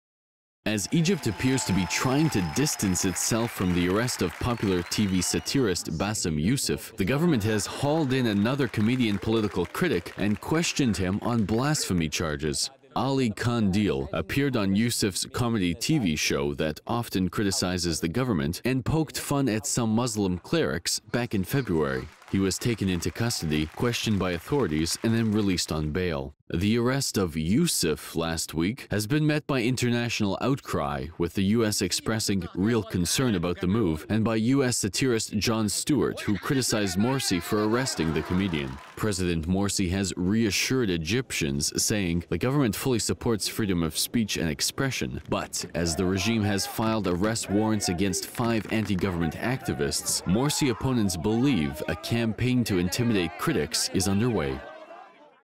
Egyptian free speech in the spotlight: second TV comic held on blasphemy charges
As Egypt appears to be trying to distance itself from the arrest of popular TV satirist Bassem Youssef, the government has hauled in another comedian political critic, and questioned him on blasphemy charges.  Ali Kandil appeared on Youssef's comedy TV show that often criticises the government and poked fun at some Muslim clerics back in February.